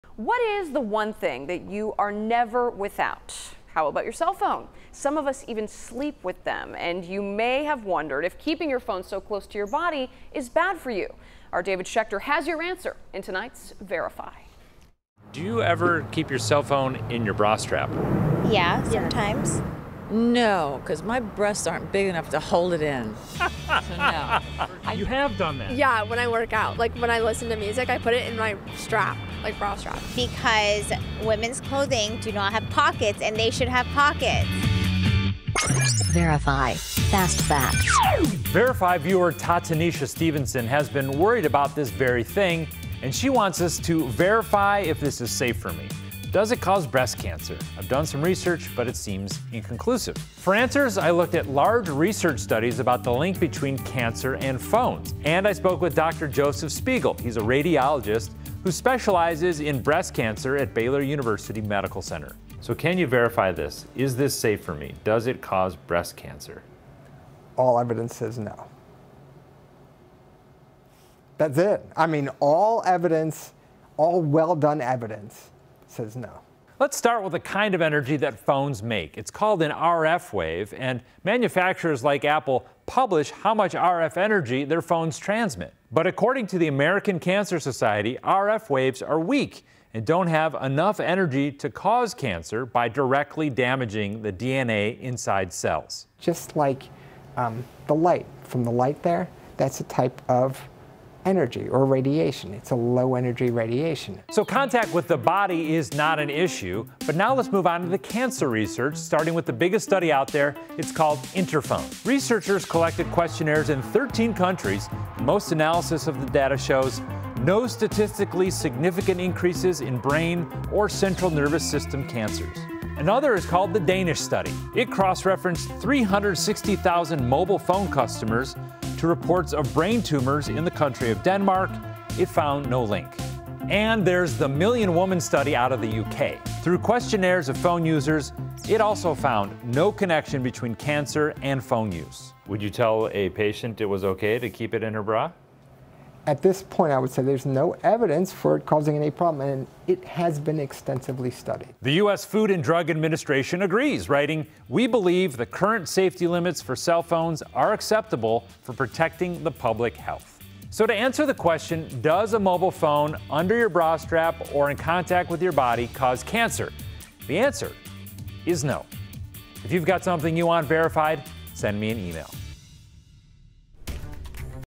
VERIFY: Can your mobile phone give you cancer?
What's the deal with phones and cancer anyway?